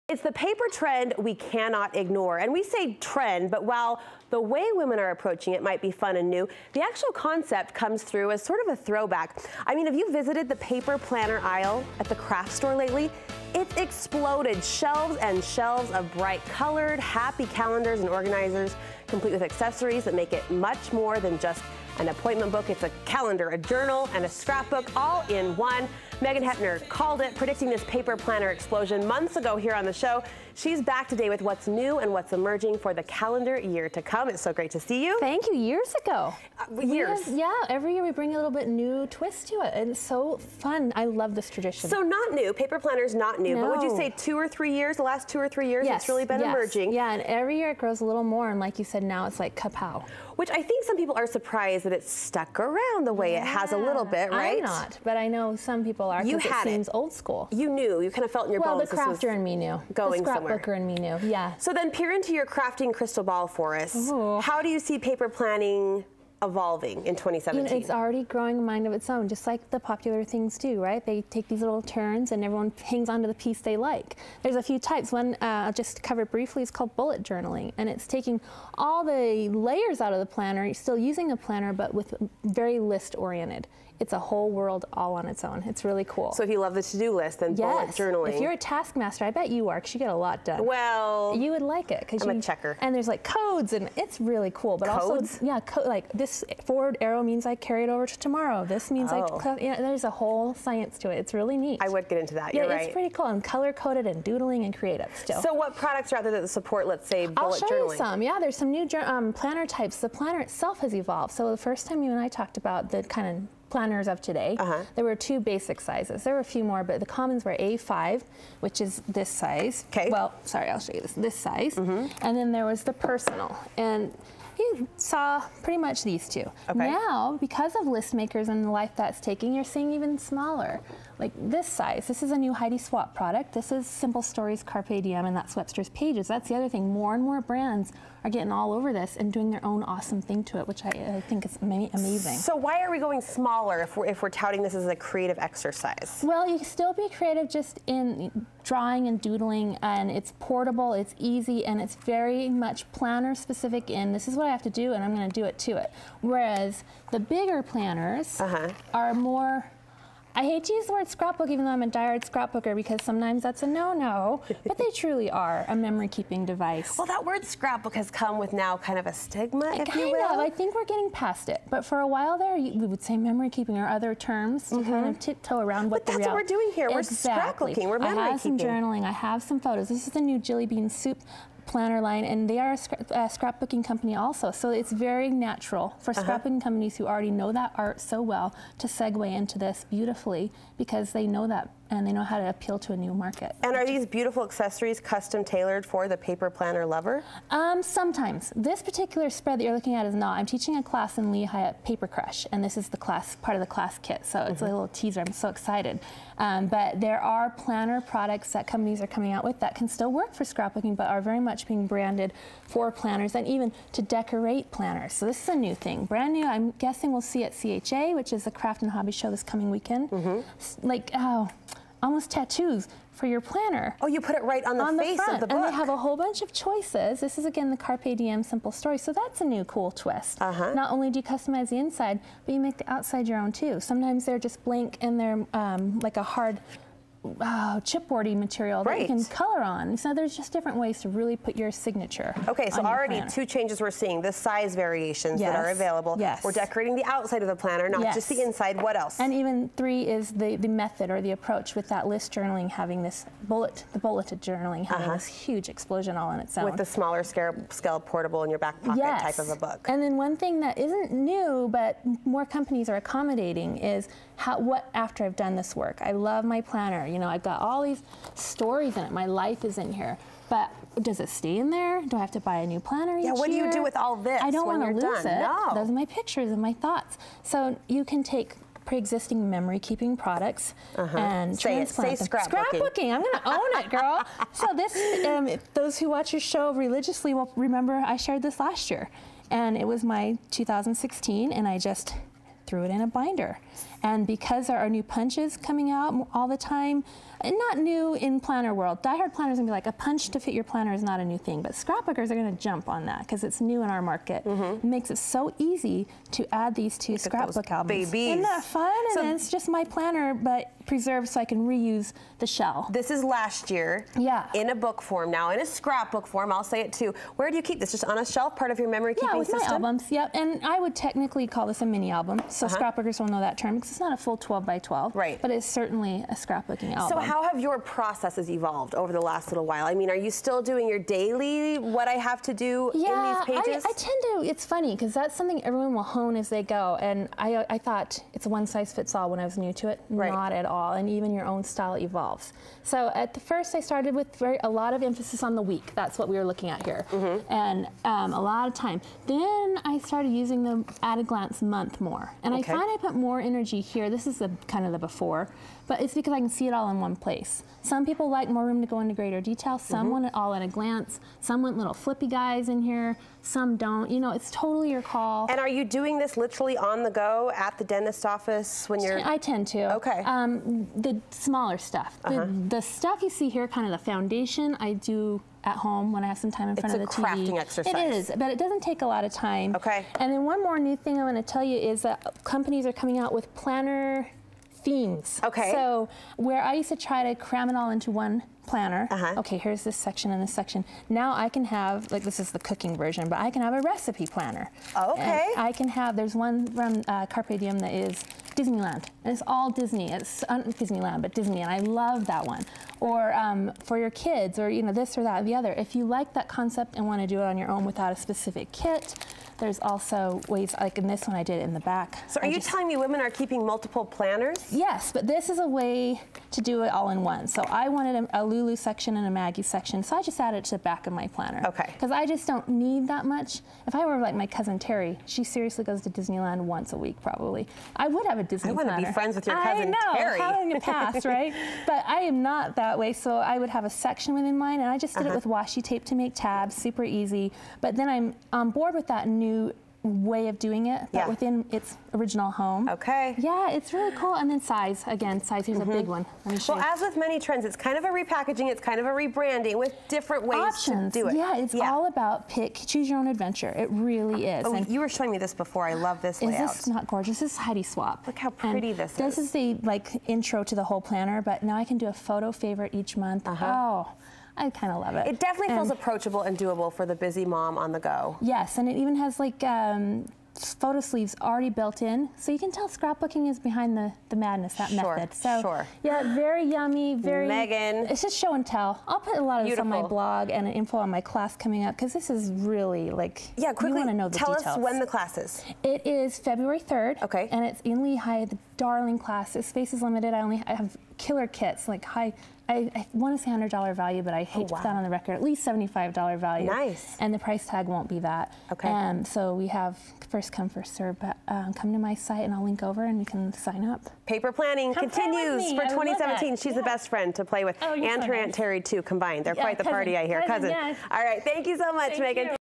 New Uses for Paper Planners
ORIGINALLY AIRED JANUARY 2017 The paper planner is turning into more than just a calendar of events. Megan Hoeppner shares what's new in paper planners and how you can use them.